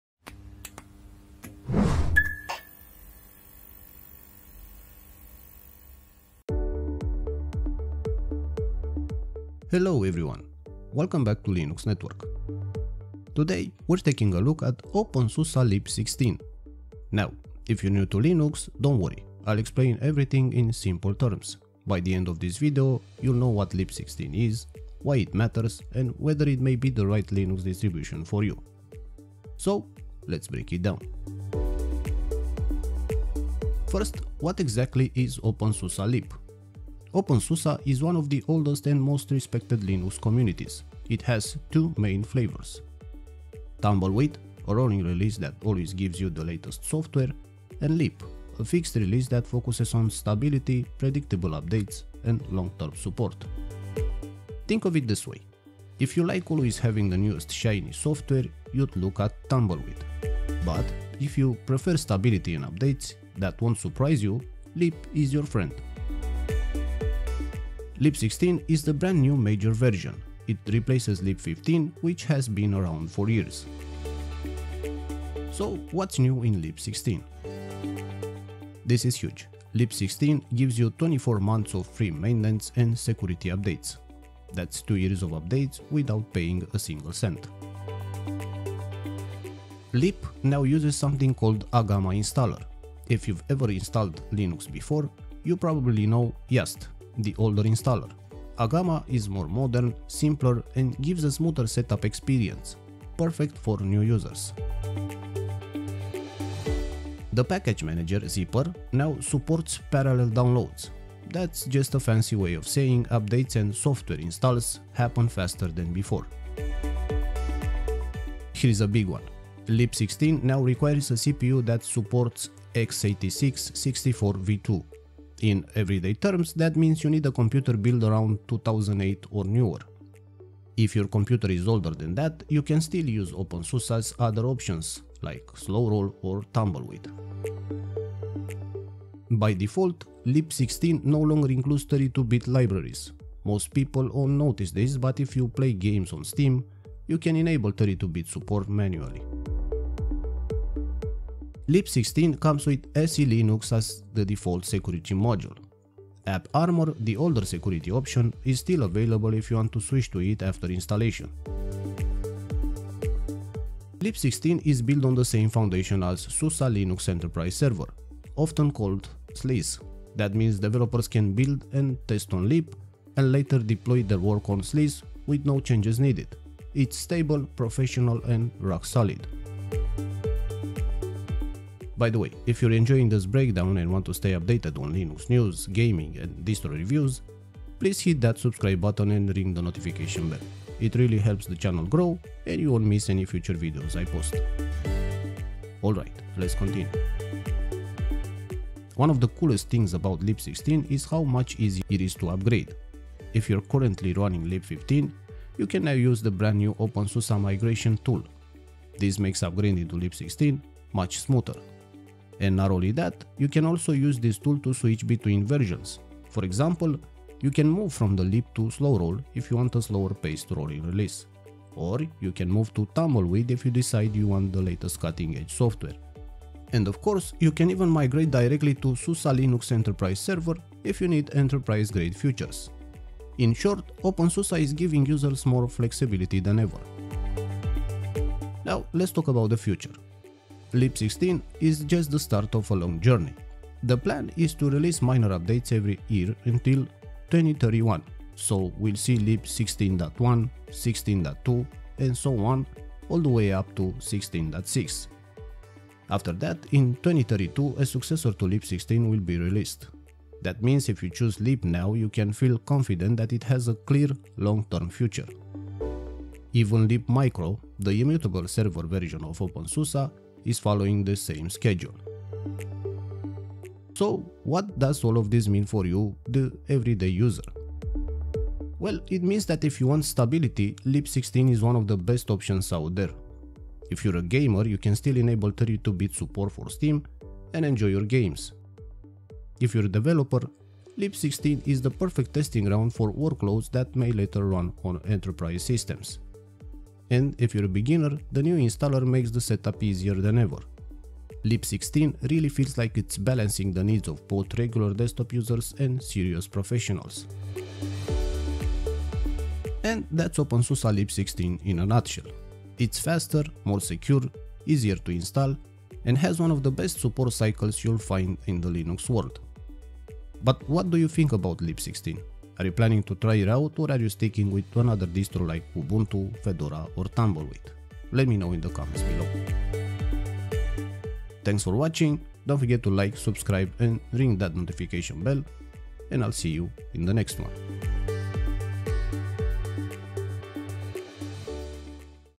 openSUSE Leap 16 | Faster, Safer, and Built for the Future!
openSUSE Leap 16 is packed with major improvements!
In this video, I’ll walk you through everything new in Leap 16, explained in simple terms for beginners. We’ll talk about the brand-new Agama installer, longer support cycle, faster updates, and why Leap 16 might just be the most stable Linux distro you can use in 2025.

Whether you’re a beginner, gamer, or developer, Leap 16 offers something for everyone, and it’s now even easier to install and maintain.